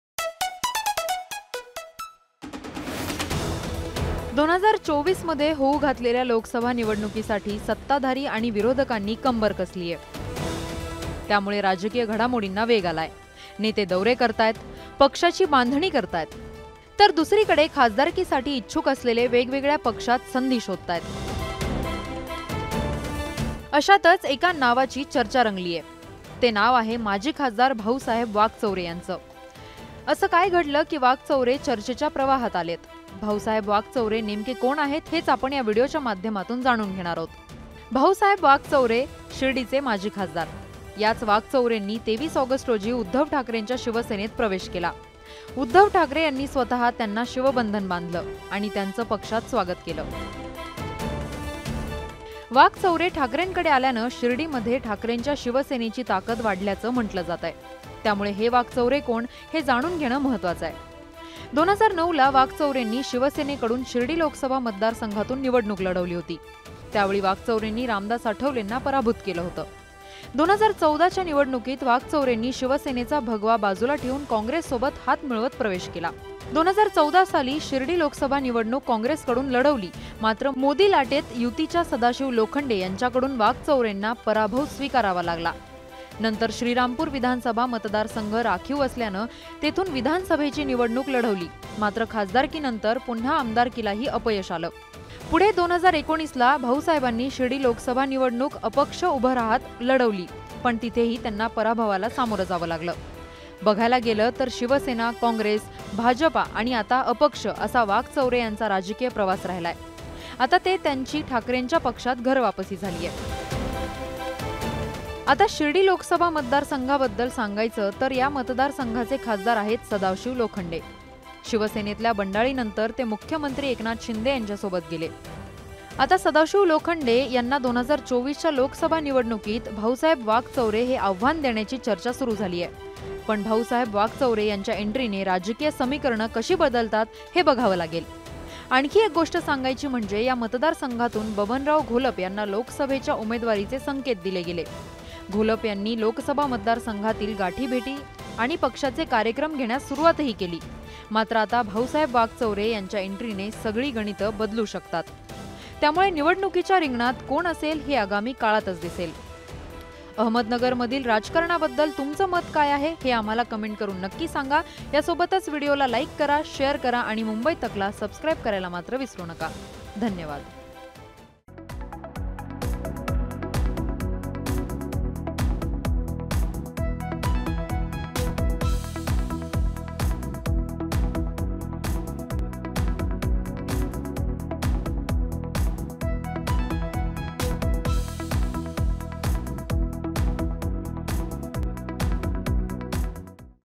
Bhausaheb Waghchaure यांची राजकीय वर्तुळात का होतेये चर्चा? | Uddhav Thackeray | Eknath Shinde
2024 मध्ये होऊ घातलेल्या लोकसभा निवडणुकीसाठी सत्ताधारी आणि विरोधकांनी कंबर कसलीये. त्यामुळे राजकीय घडामोडींना वेग आलाय. नेते दौरे करताहेत. पक्षाची बांधणी करताहेत. तर दुसरीकडे खासदारीसाठी इच्छुक असलेले वेगवेगळ्या पक्षात संधी शोधताहेत. अशातच एका नावाची चर्चा चांगलीच रंगलीये. ते नाव आहे माजी खासदार भाऊसाहेब वाकचौरे यांचं. असं काय घडलं की वाकचौरे चर्चेच्या प्रवाहात आलेत. भाऊसाहेब वाकचौरे नेमके कोण आहेत? हेच आपण या व्हिडिओत जाणून घेणार आहोत.